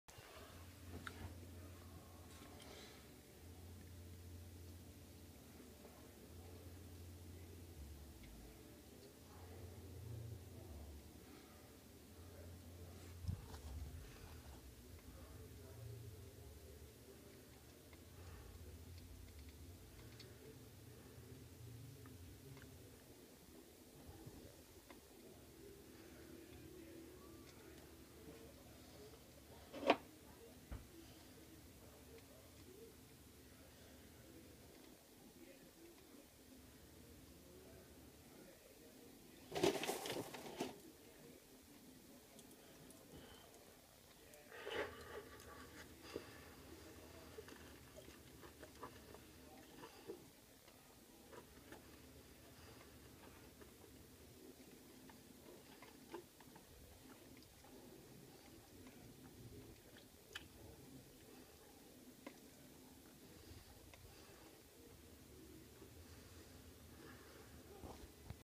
demon perhaps ?
ugly entity in my sister's  back yard. pay attention to the fire and what's standing behind it